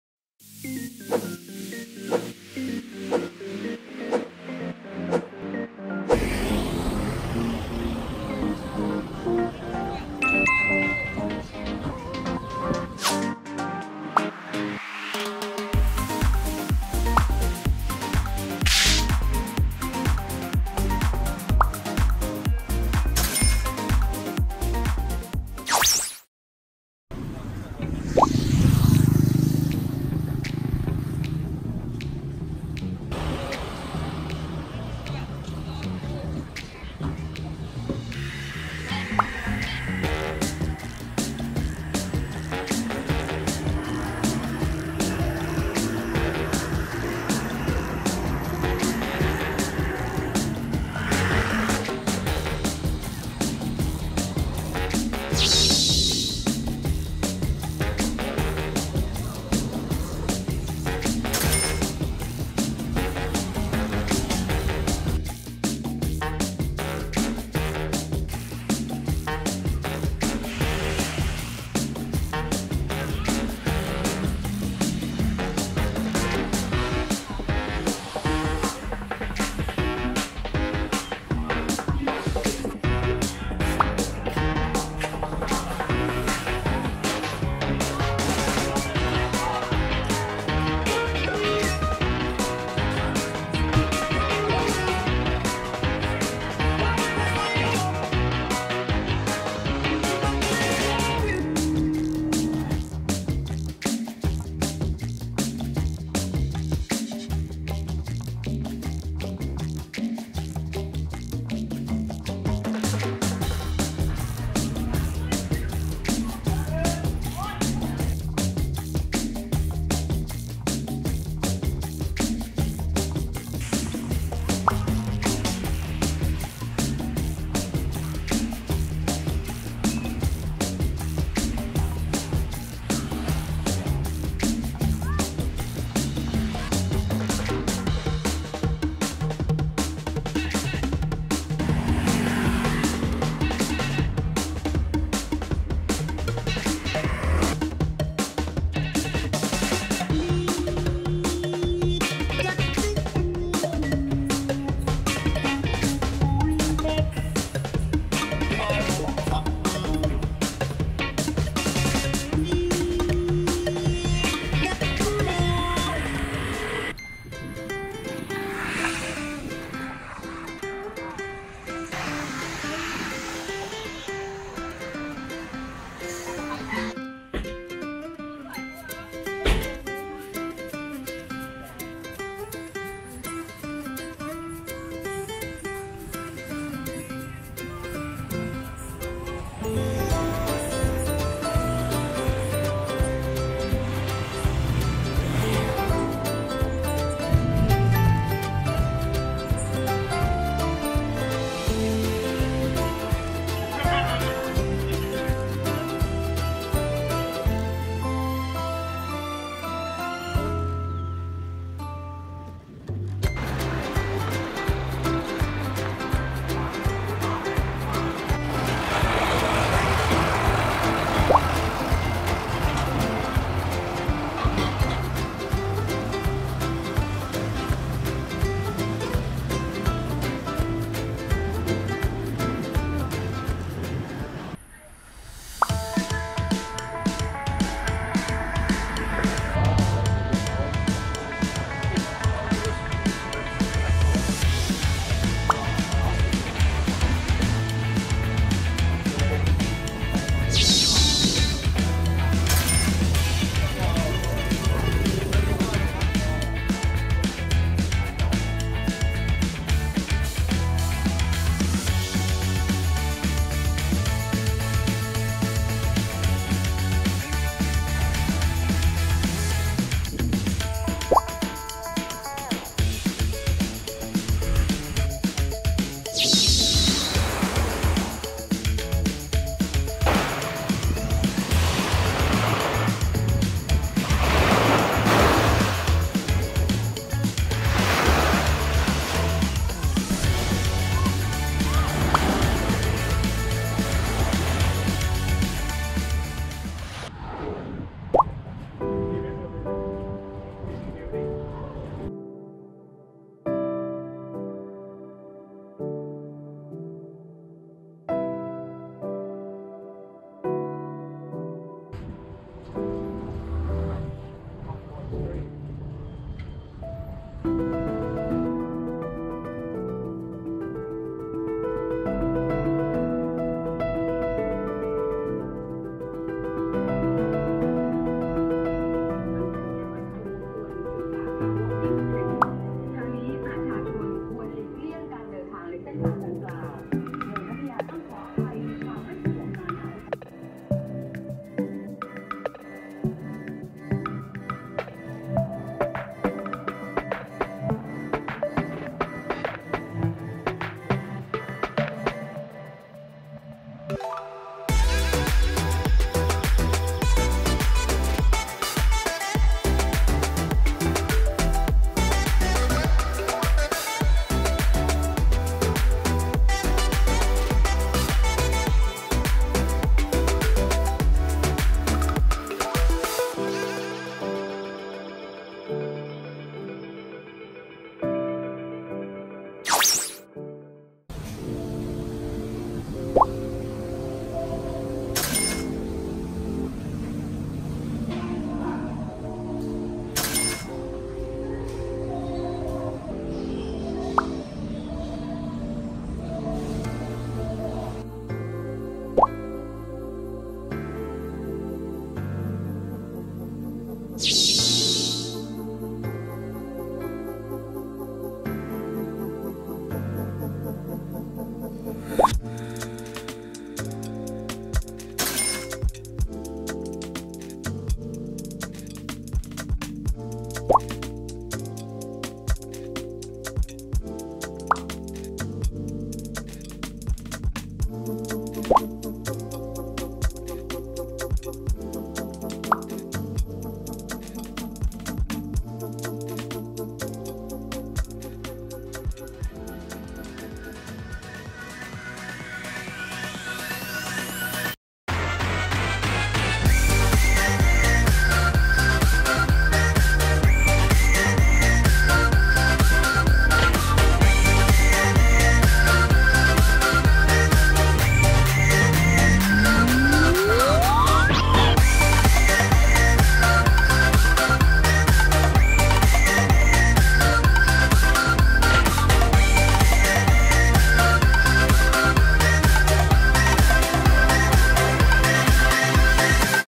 Jomtien Beach in Pattaya was great with a calm atmosphere -Travelog-
* The video was taken in May 2019, before the Corona disaster.

This video is a Vlog
that chill out at
Jomtien Beach in Pattaya.